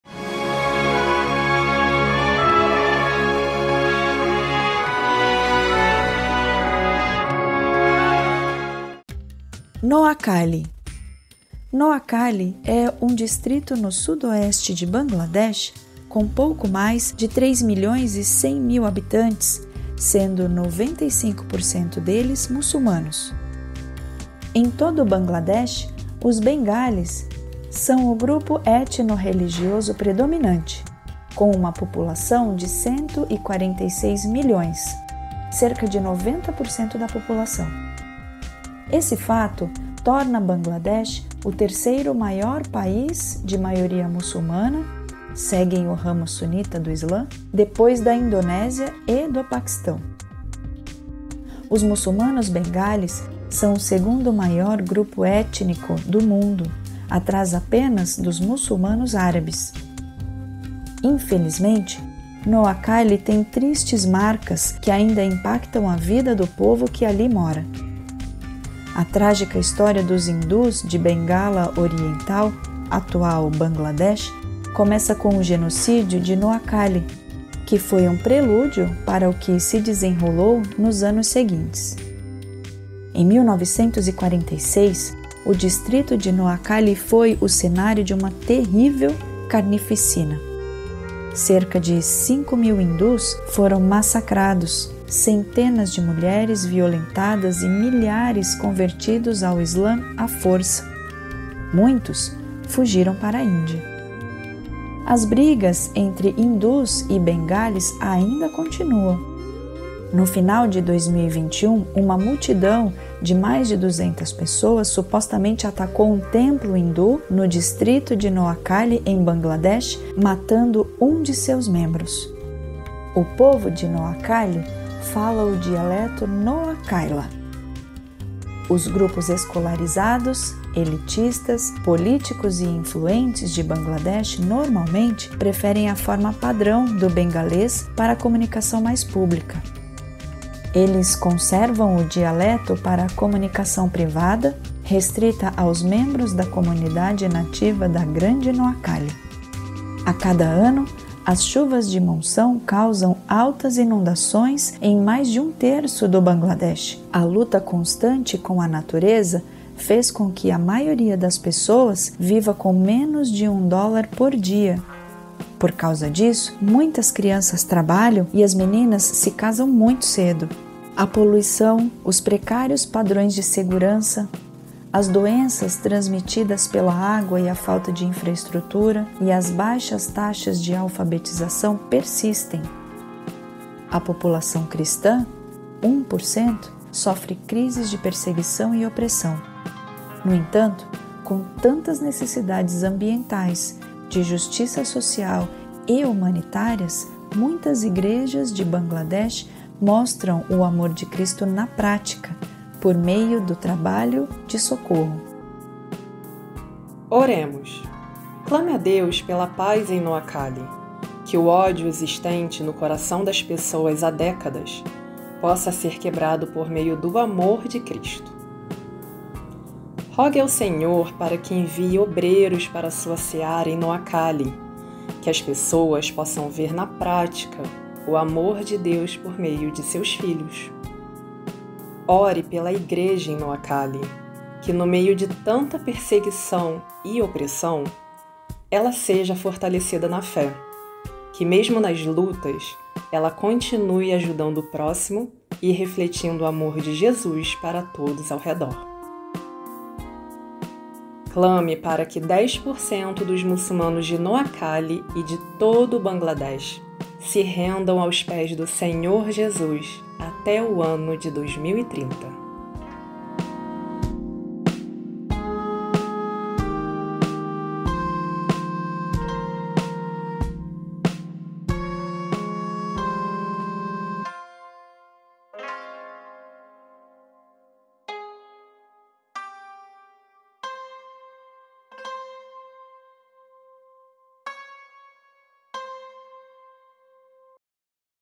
ROTA 10/10 2024 #8 NOAKHALI, Bangladesh (ORAÇÃO PELOS POVOS MUÇULMANOS)
Noakhali é um distrito no sudoeste de Bangladesh, com pouco mais de 3 milhões e 100 mil habitantes, sendo 95% deles muçulmanos.
Em todo o Bangladesh, os bengalis são o grupo etno-religioso predominante, com 146 milhões de habitantes (cerca de 90% da população). Esse fato torna Bangladesh o terceiro maior país de maioria muçulmana (seguem o ramo sunita do Islã), depois da Indonésia e do Paquistão. Os muçulmanos bengalis são o segundo maior grupo étnico do mundo, atrás apenas dos muçulmanos árabes.

A população cristã (1%) sofre crises de perseguição e opressão. No entanto, com tantas necessidades ambientais, de justiça social e humanitárias, muitas igrejas de Bangladesh mostram o amor de Cristo na prática, por meio do trabalho de socorro.

OREMOS:

●  Clame a Deus pela paz em Noakhali; que o ódio existente no coração das pessoas há décadas possa ser quebrado por meio do amor de Cristo;

●  Rogue ao Senhor para que envie obreiros para a Sua seara em Noakhali. Que as pessoas possam ver, na prática, o amor de Deus por meio de Seus filhos;

●  Ore pela igreja em Noakhali. Que no meio de tanta perseguição e opressão, ela se fortaleça na fé em Cristo; que, mesmo nas lutas, ela continue ajudando ao próximo e refletindo o amor de Cristo para todos ao redor;

●  Que 10% dos muçulmanos de Noakhali e de todo o Bangladesh se rendam aos pés de Jesus até o ano de 2030.